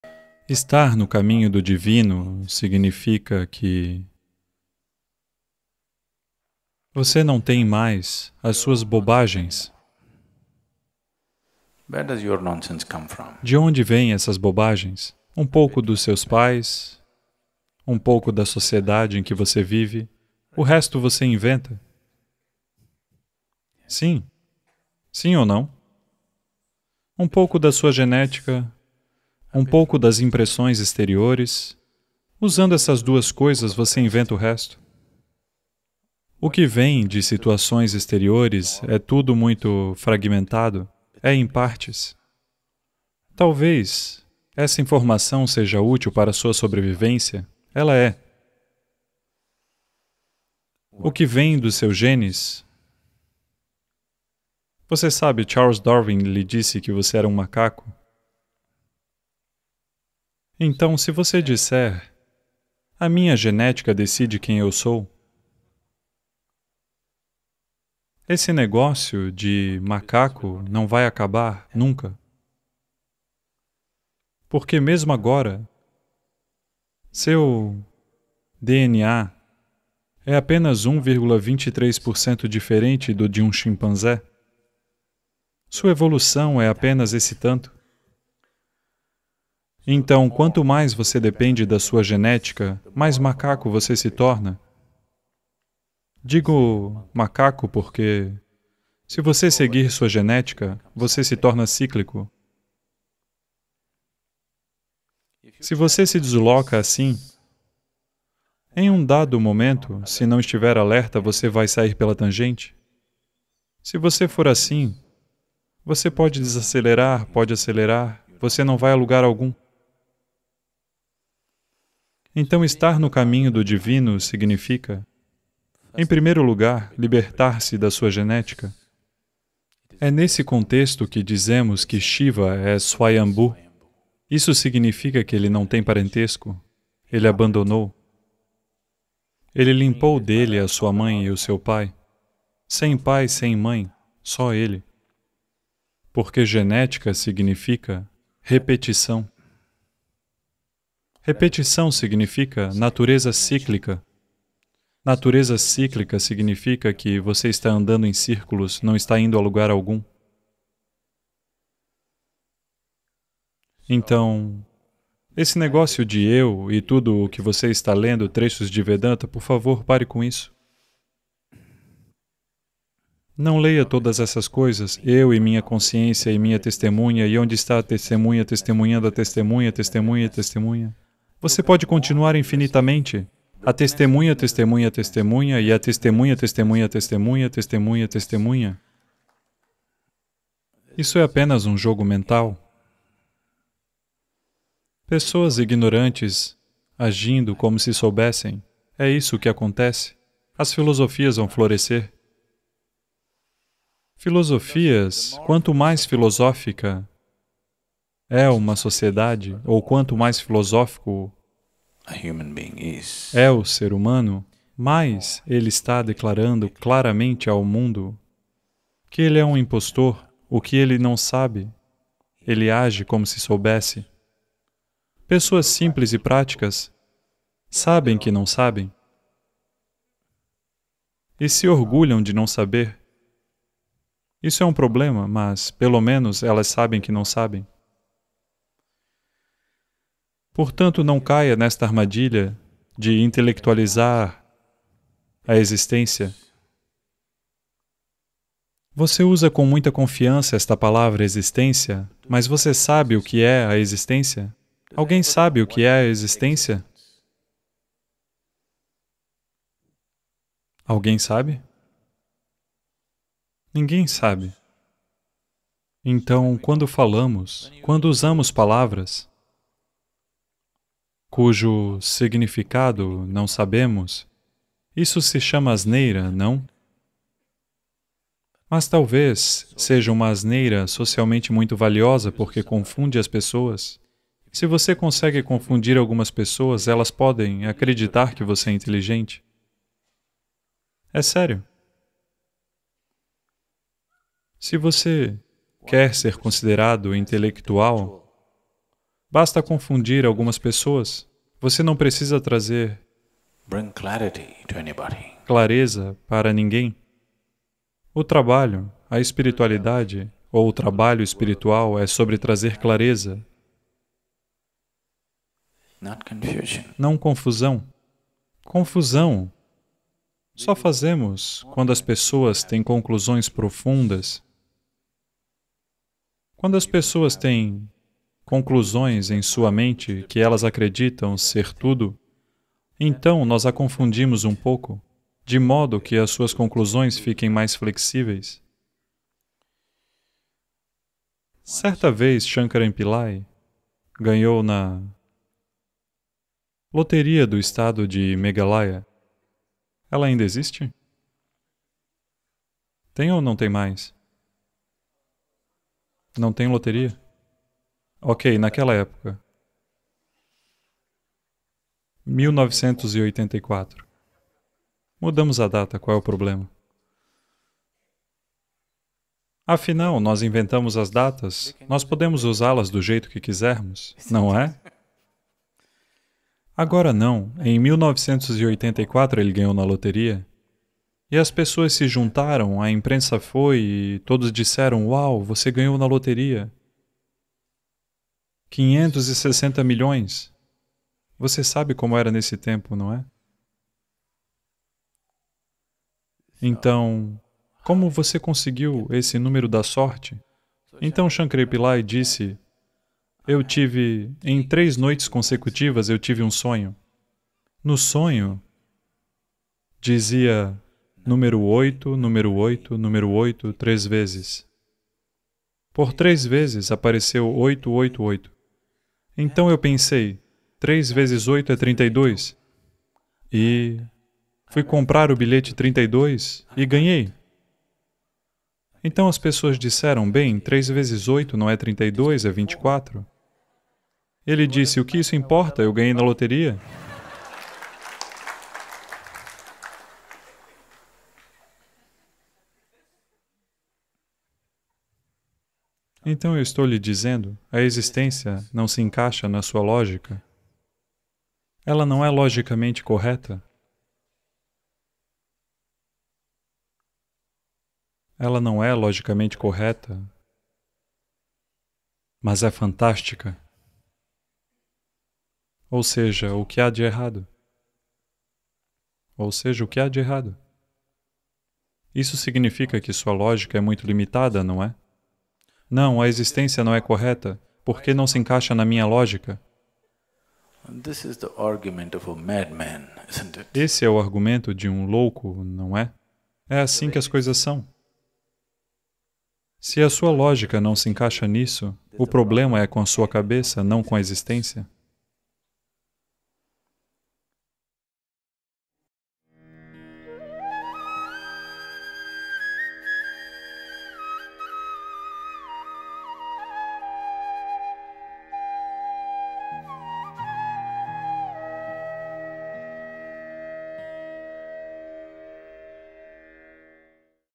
A Existência Não é Lógica, Abandone Suas Bobagens | Sadhguru Português
Frustrado que as coisas não estão indo do jeito que você acha que deveriam acontecer? Sadhguru explica que, quando você começa a pensar "não, a existência não é correta, por que não se encaixa na minha lógica", esse é o argumento de um louco. Ele aponta que a existência não é lógica, mas é absolutamente fantástica.